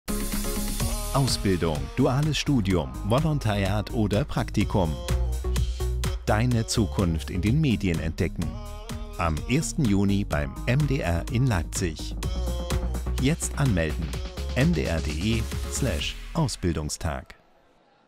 Komm zum 9. Ausbildungstag von MDR und MDR Media
Medienberufe hautnah erleben und ausprobieren: Am 1. Juni 2024 laden MDR und MDR Media zum 9. Ausbildungstag ein. Schülerinnen und Schüler ab Klasse 8 sowie deren Familien können sich von 10 bis 15 Uhr im Foyer des MDR-Produktionshochhauses in der Leipziger Kantstraße über neun verschiedene Ausbildungen, duales Studium, Redaktionsvolontariat, Praktika sowie Jobs an Praxisinseln, bei Führungen und zahlreichen Workshops umfassend informieren.

Melde dich jetzt hier an